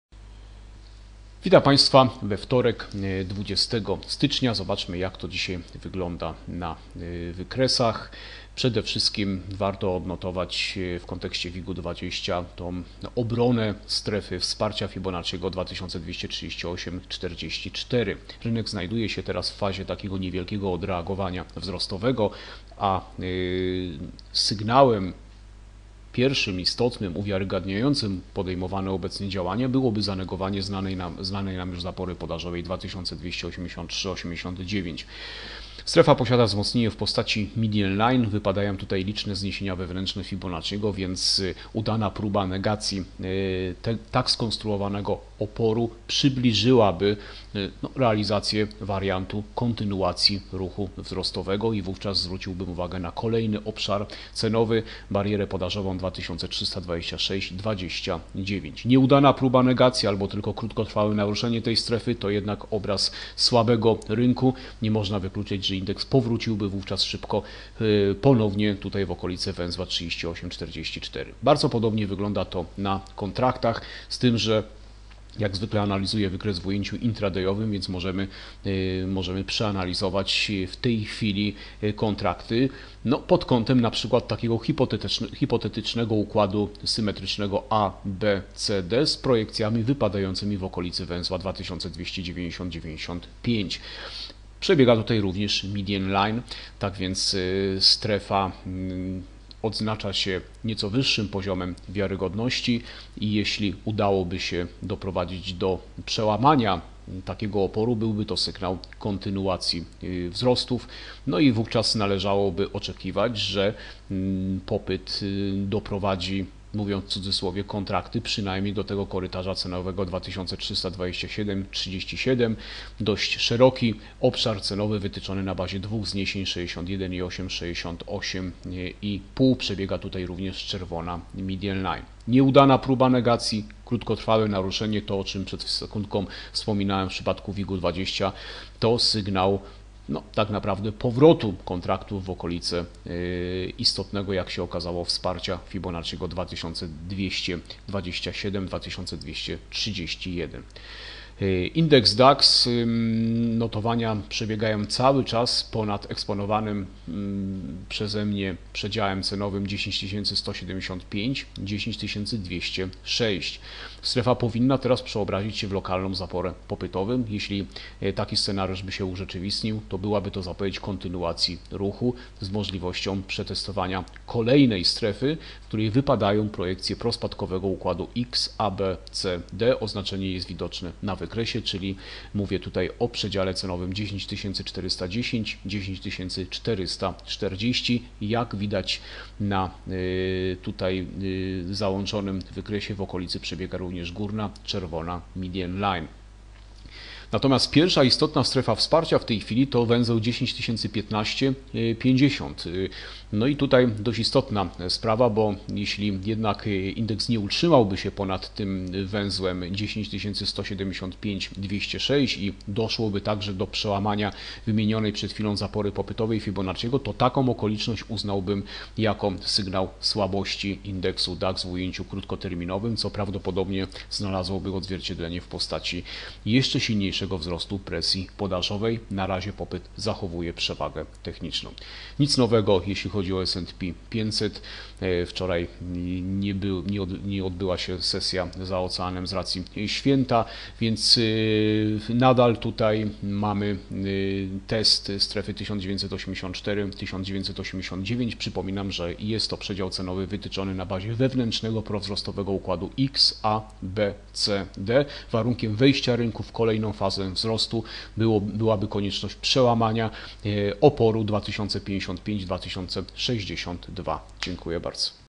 Analiza WIG20, DAX i S&P500 - 20.01.2015
Obejrzyj nasz materiał wideo "Analiza indeksów: WIG20, DAX i S&P500". Znajdziesz w nim komentarz Pawła Danielewicza dotyczący wybranych indeksów giełdowych.